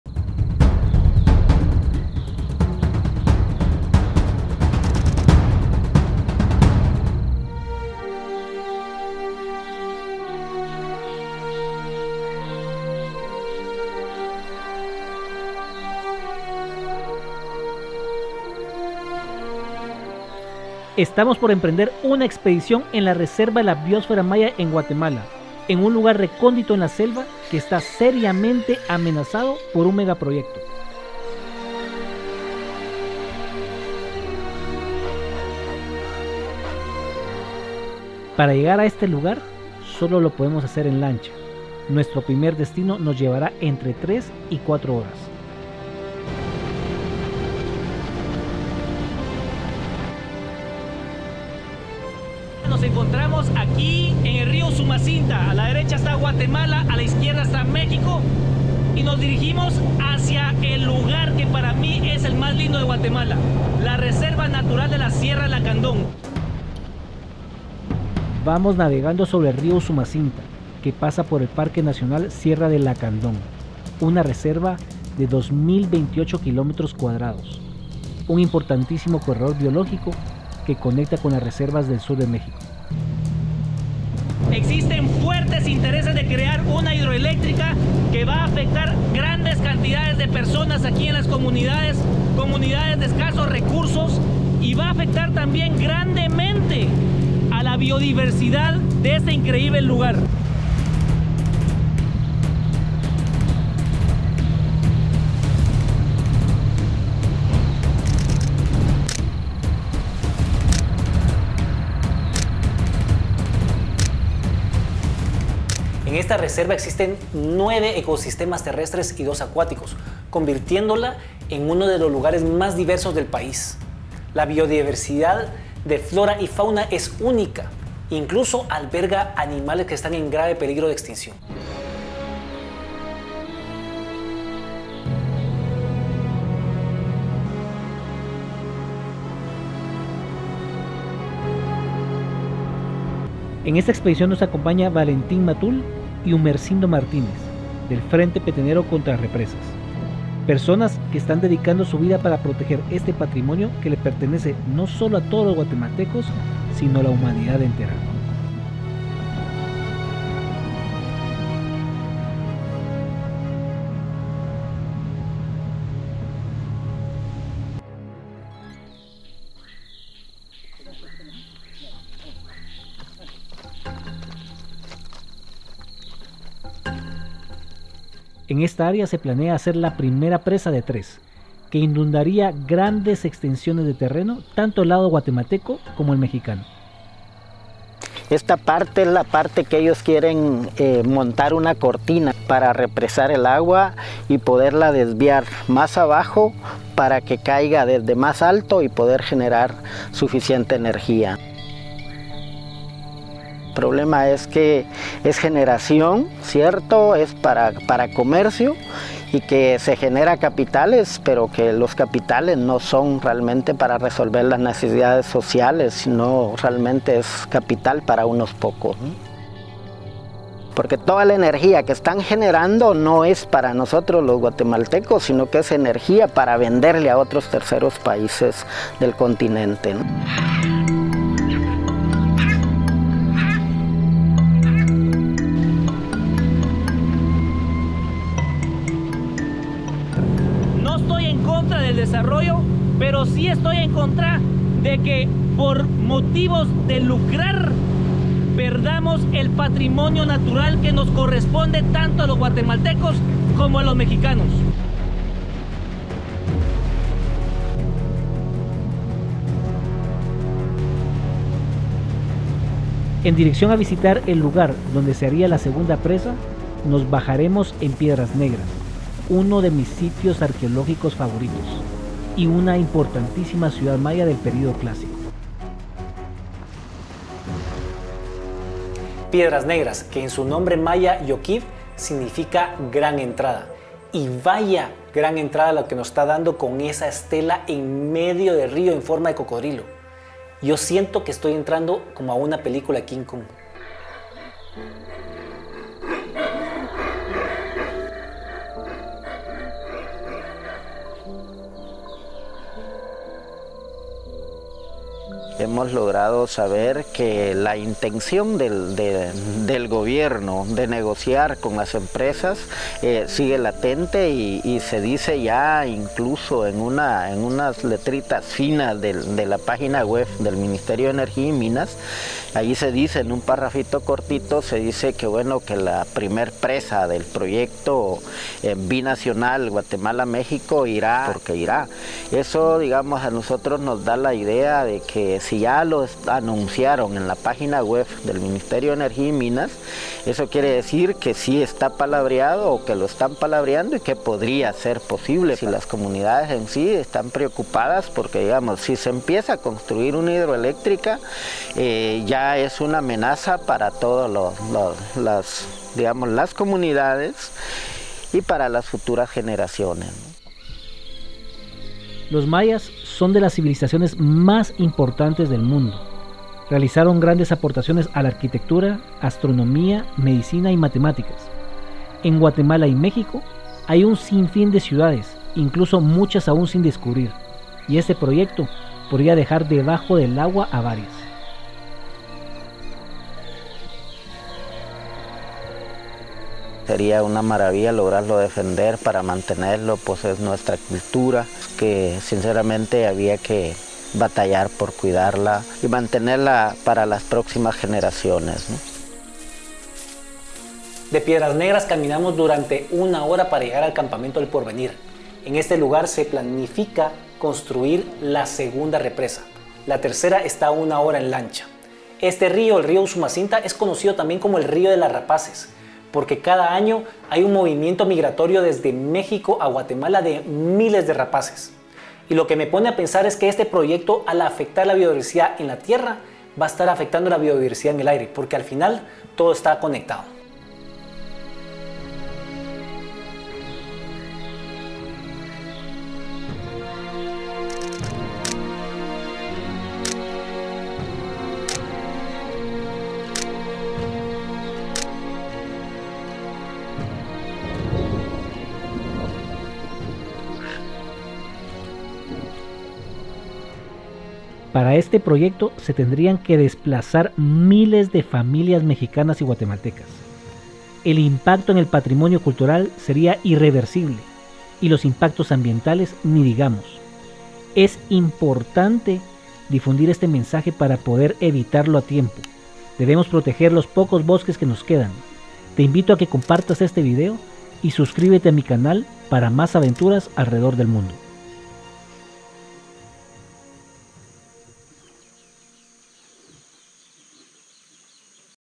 DESTRUCCIÓN de la SELVA una razón que DESCONOCÍA
En la frontera de México y Guatemala existe una amenaza que atentaría con la destrucción de una inmensa extención de los remanentes bosques en la reserva natural de la biósfera Maya en frontera con México (Chiapas) en el río Usumacinta.